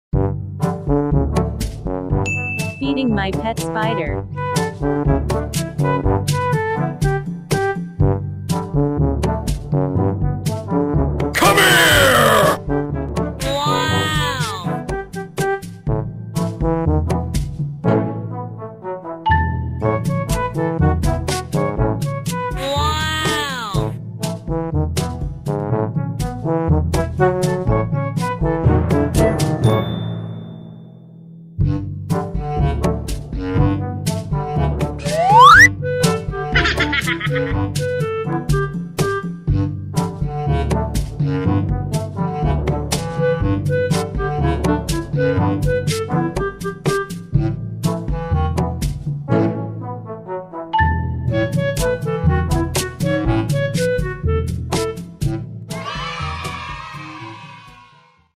Feeding my Pet Spider: Neoscona vigilans. Gagambang Tigre
Feeding our Pet Spider, Neoscona viligans or Gagambang Tigre from Cebu..